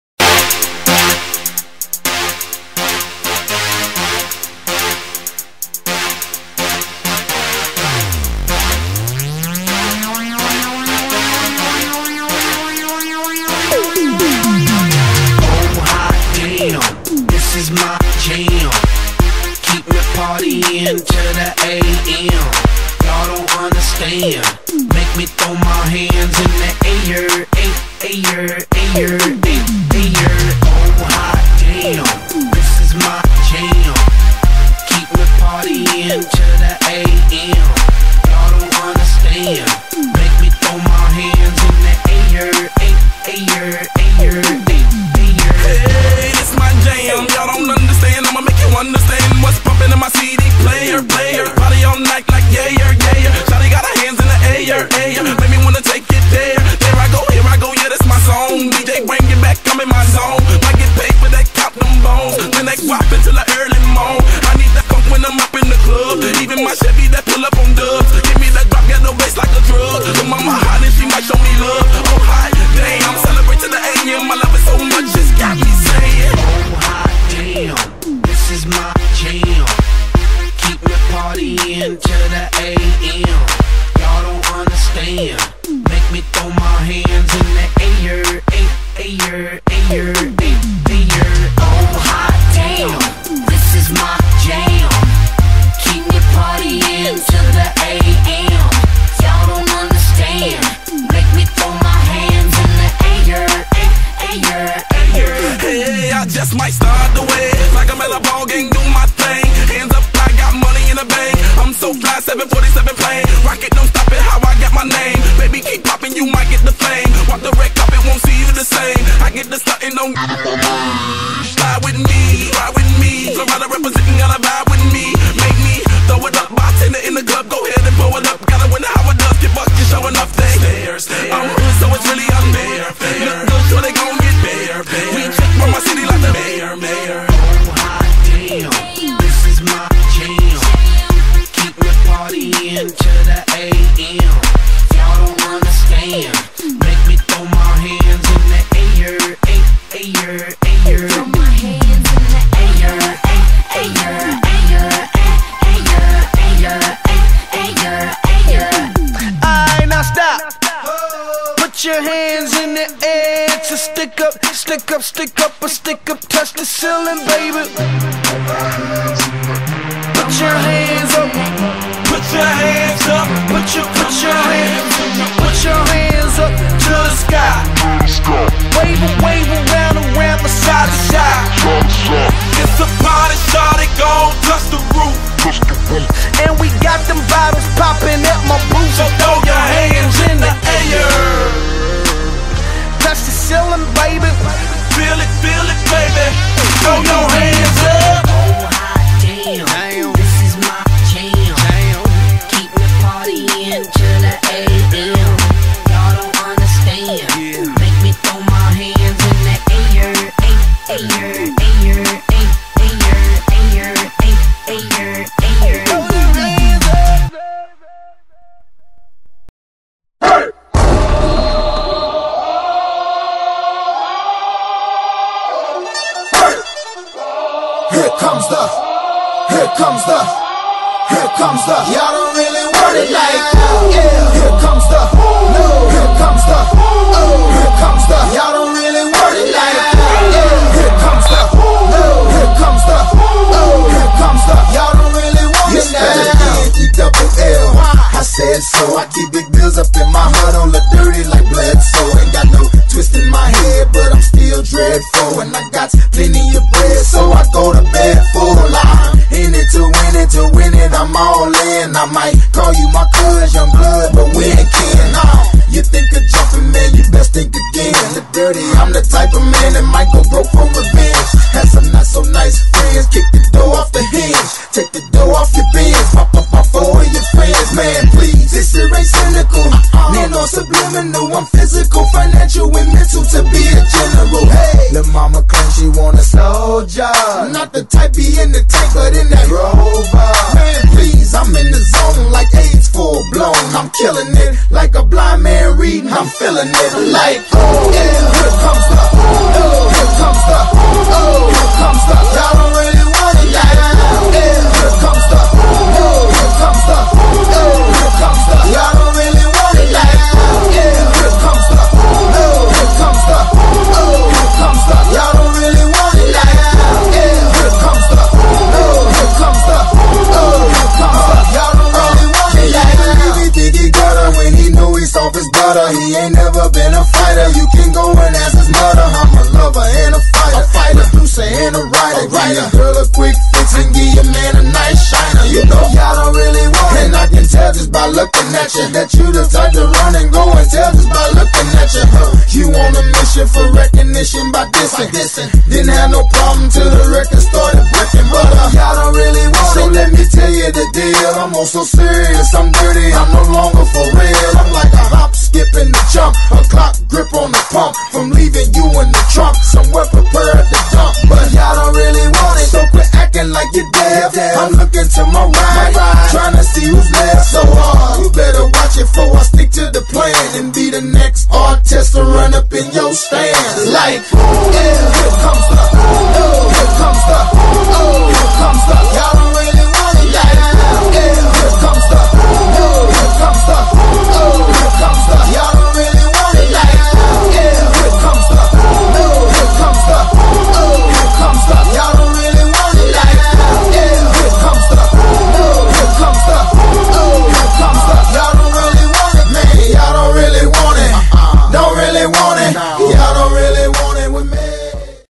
Halls Football Montage
Video montage of the Hall's Red Devils 2008 Season!